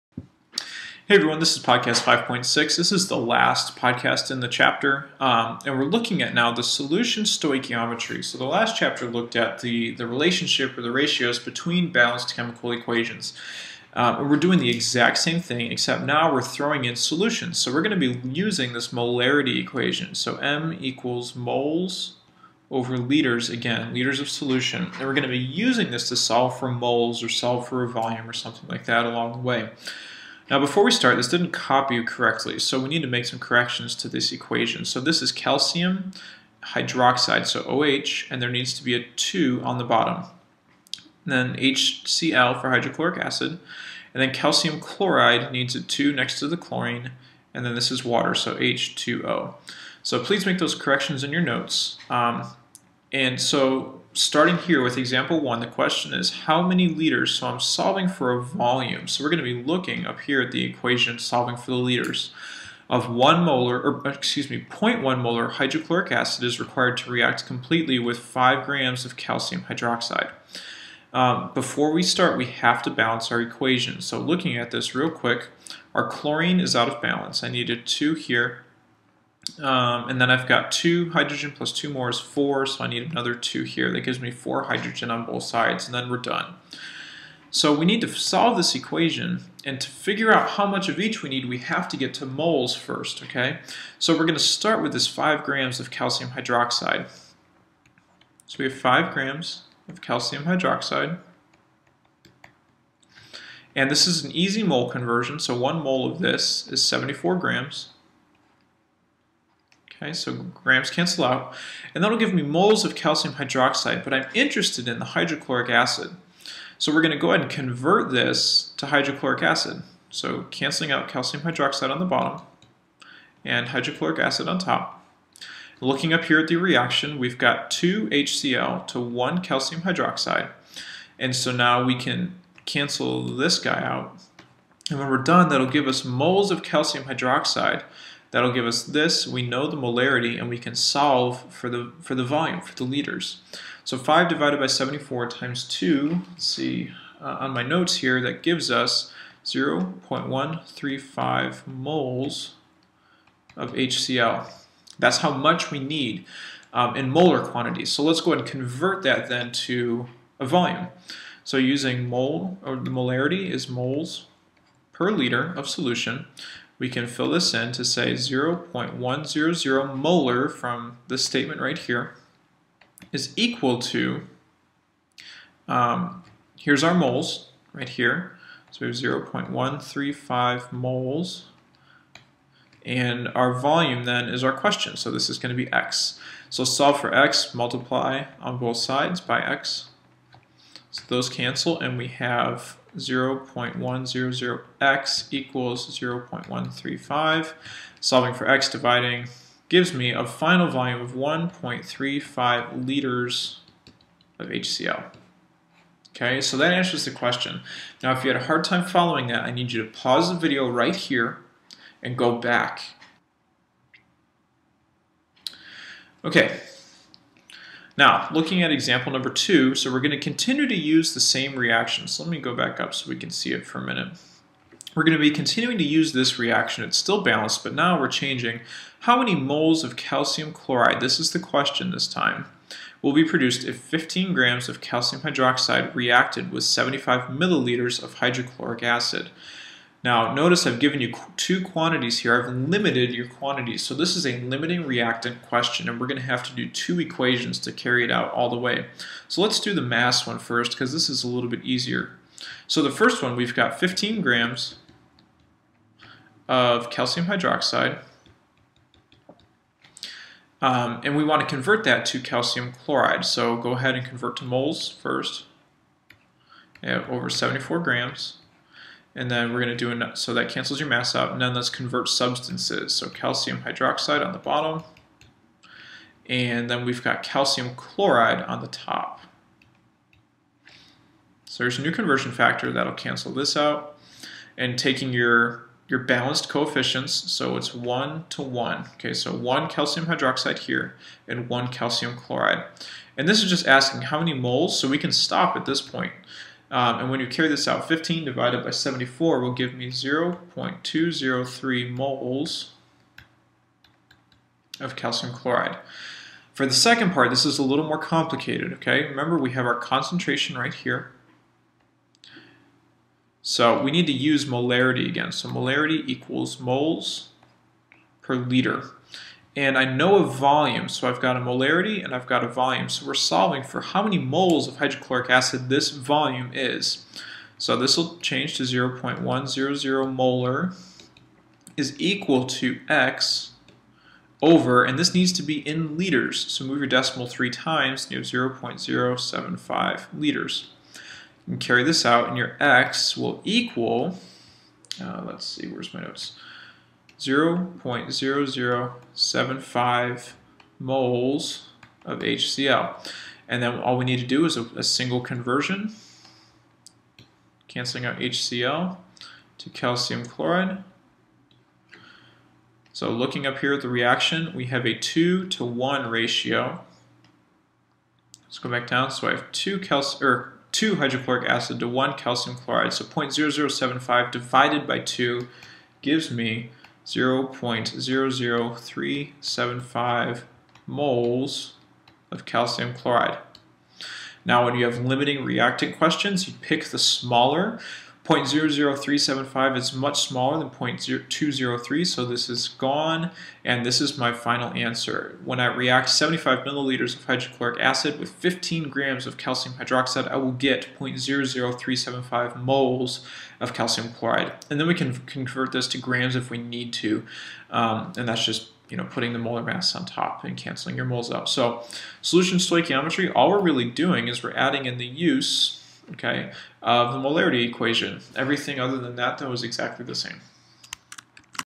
5.6 Solution Stoichiometry
Molarity can also be used when performing stoichiometric conversions that include aqueous components.